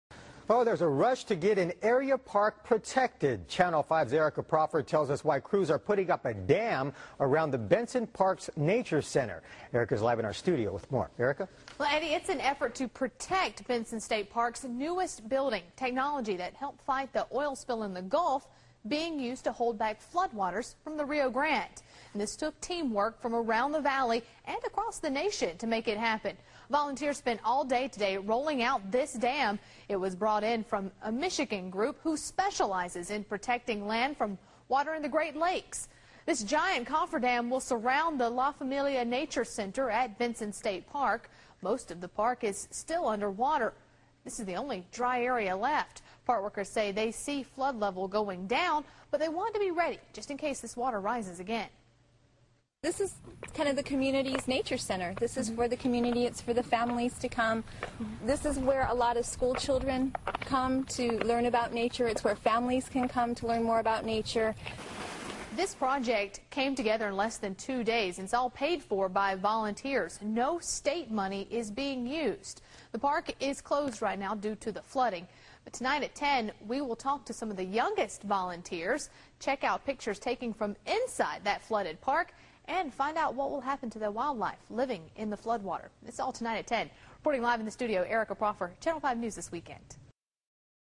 cofferdams
New water dam technology helps a community
protect a park building from a flood.